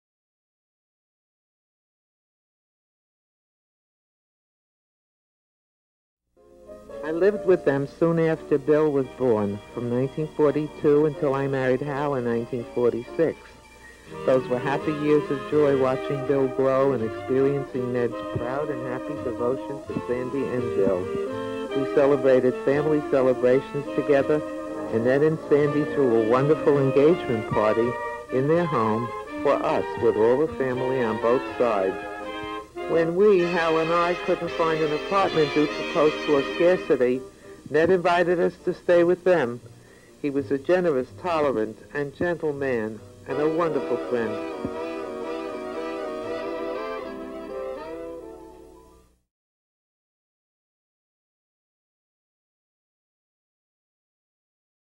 BILL’S EARLY LIFE
Aunt Margo talks about Bill’s early life, and we see photos.